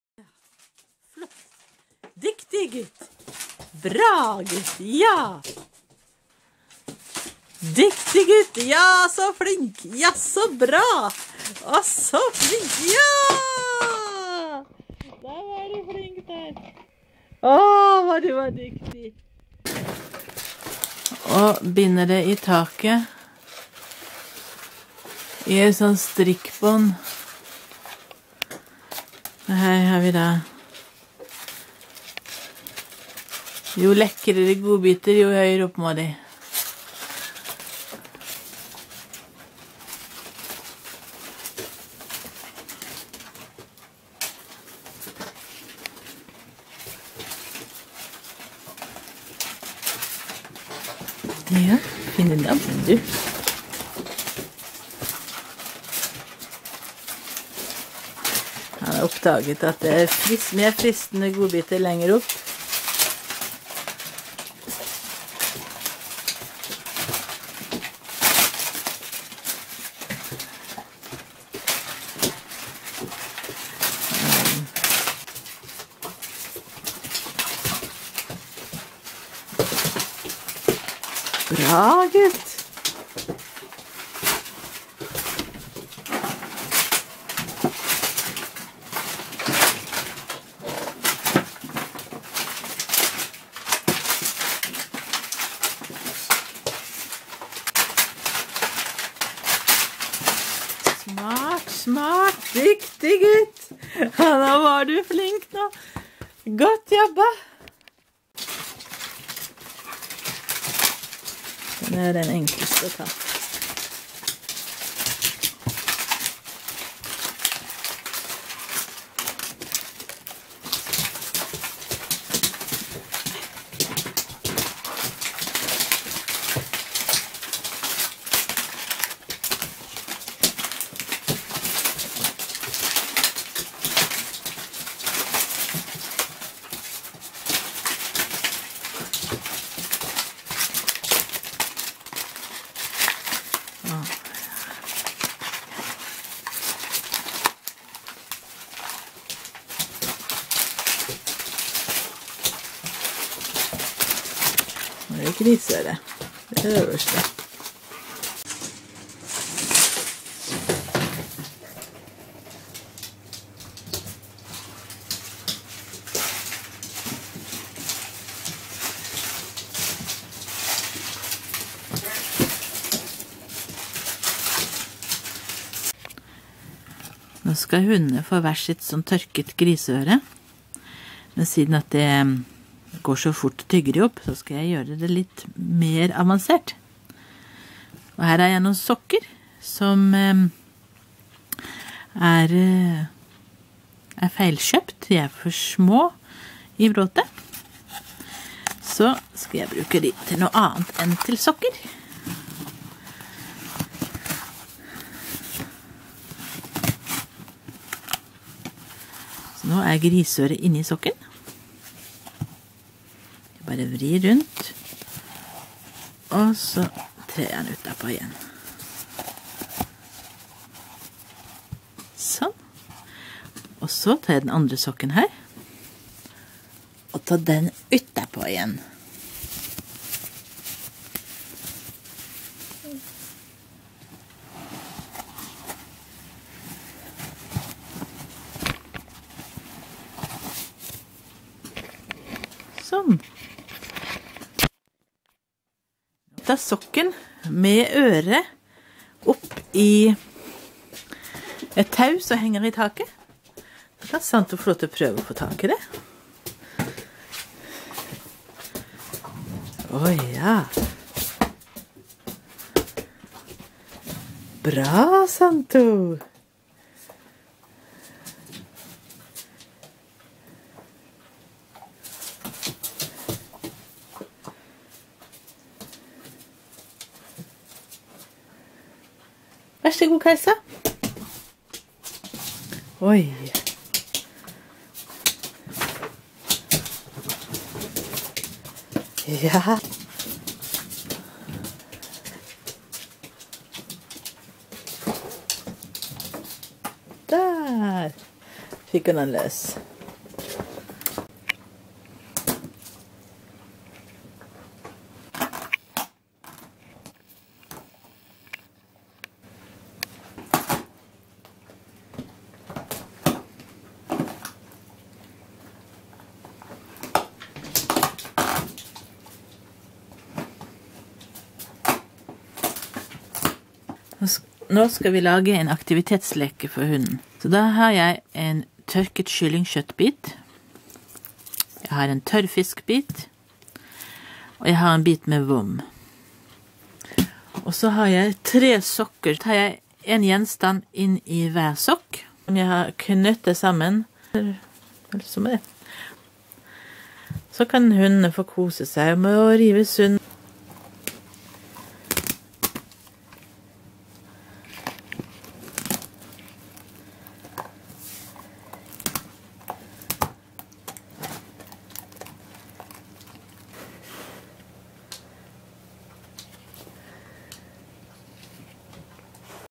Innpakka godbiter, med en liten vri.
Her er helt enkel aktivisering av hunden, som krever minimalt av eier. Kan være flott å bruke som en belønning under trening, når hunden er ekstra flink, eller som ren lett aktivisering når eier er syk, og ikke kan aktivisere hunden like mye som vanlig. Flere filmer ser du på nettsiden